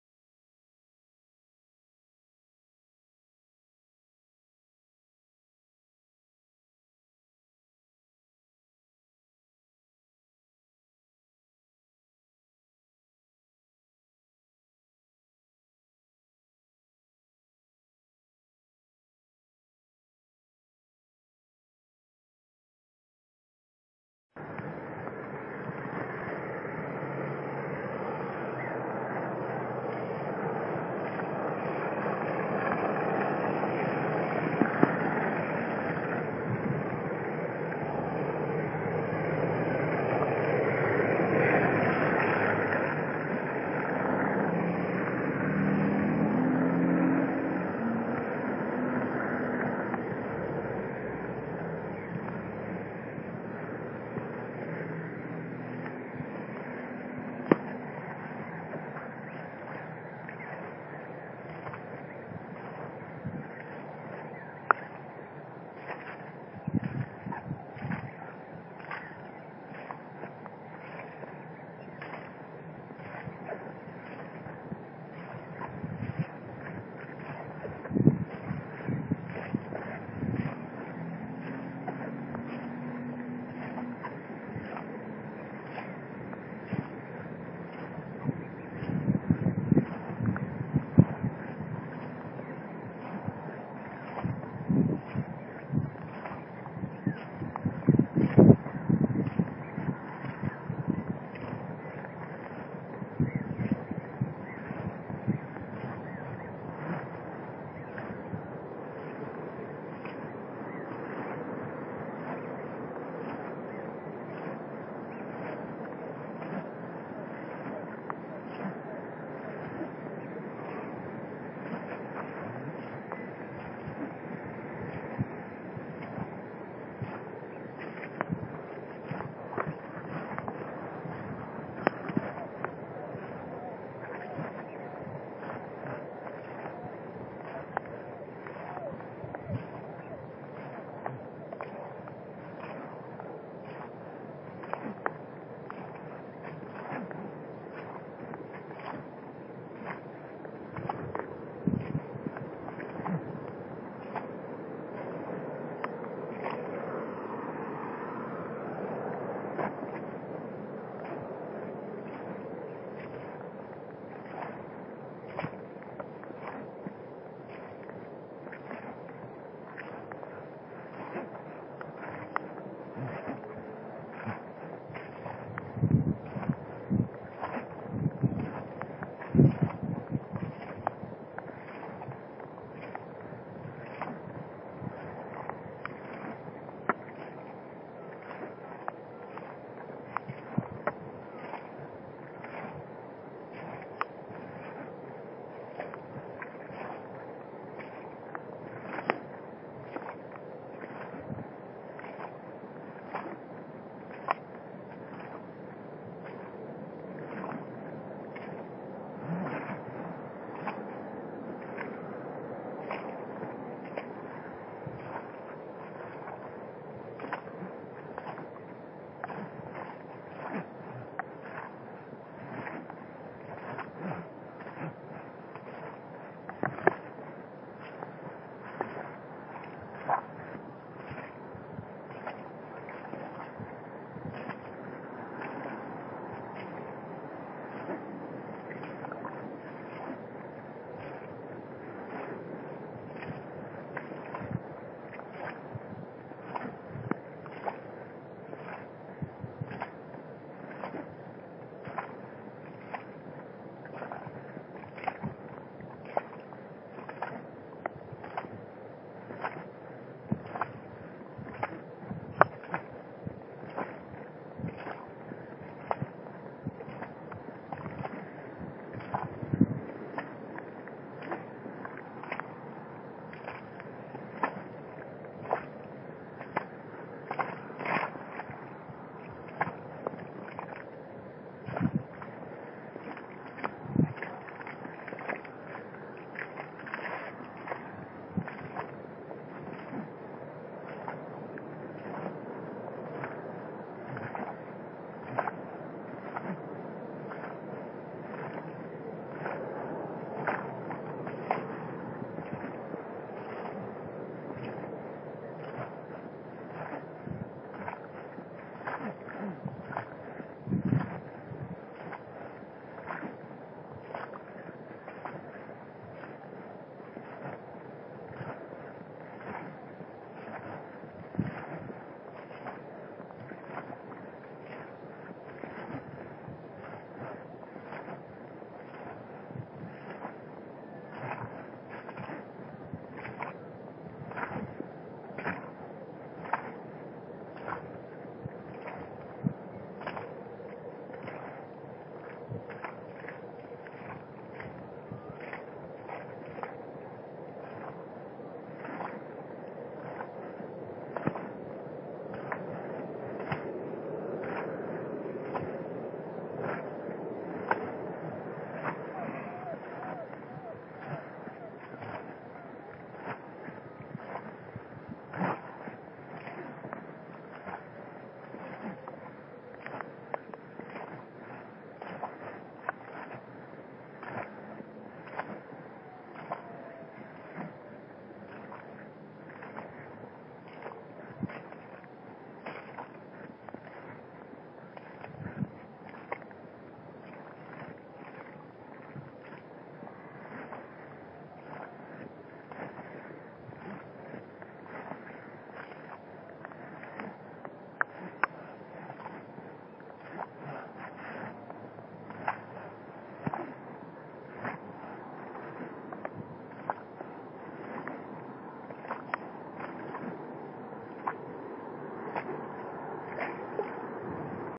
Rockport Boots Muddy Field Walk
It rained very heavily in the night and while I was walking so thought I would wear my Rockports to work, and I din't get wet.